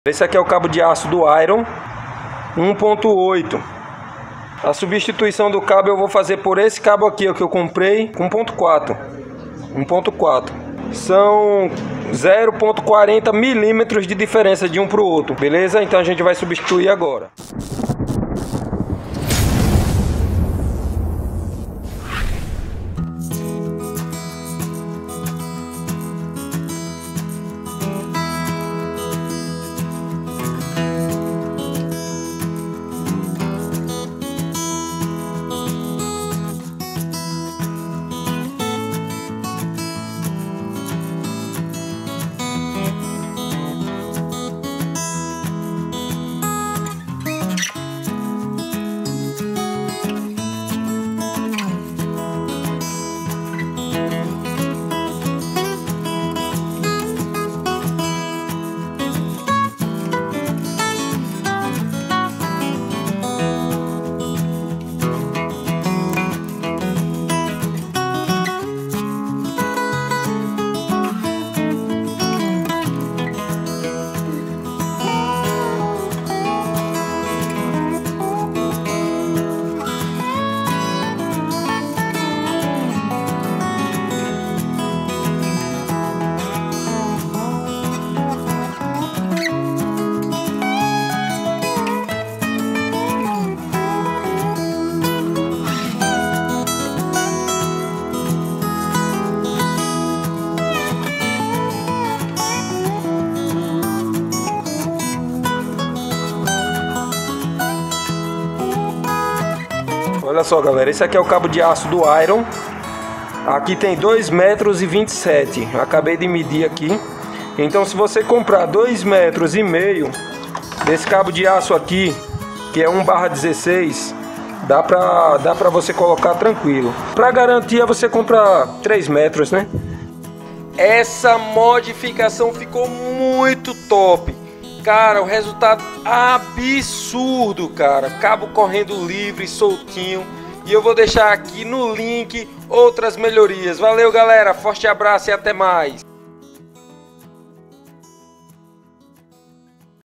Trocando Cabo de Aço Caiaque Iron
Nesse vídeo eu faço a substituição do cabo de aço do caiaque Iron que na minha opinião veio muito grosso e estava travando no conduite, esse foi um dos motivos pelo qual eu perdi o controle do leme logo no meu primeiro dia de uso do caiaque Iron.

Cabo de aço original:  2mm
Cabo de aço que coloquei:  1,6mm